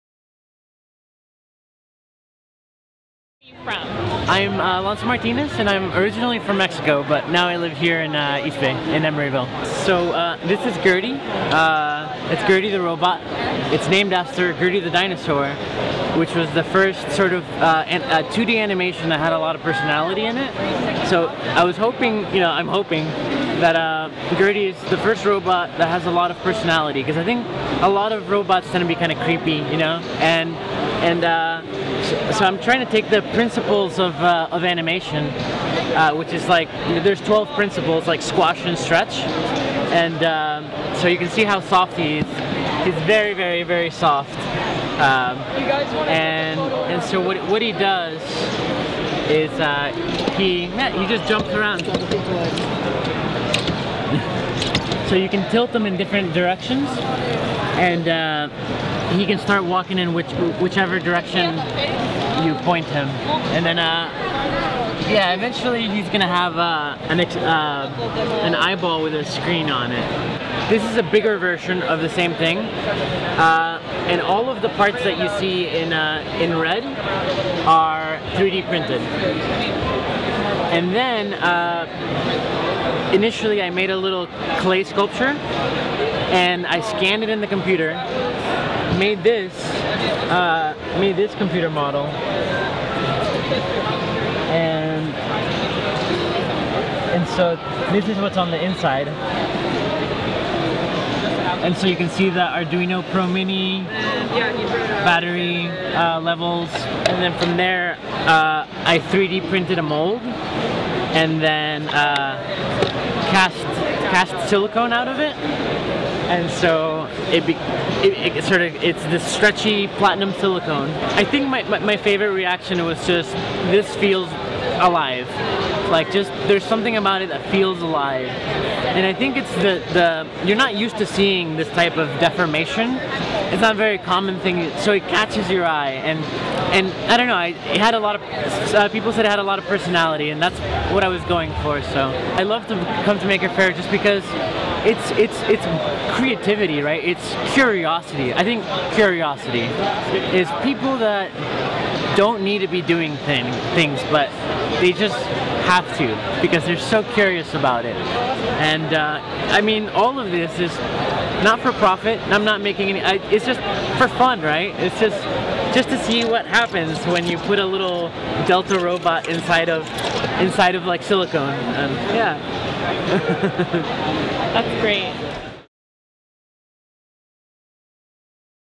Gertie the Jumping Robot
Gertie is an egg-like robot that moves around by jumping! His outside is make out of silicone so that it squashes and stretches as it moves around. Swing by and button mash the hacked wireless buttons and watch them jump!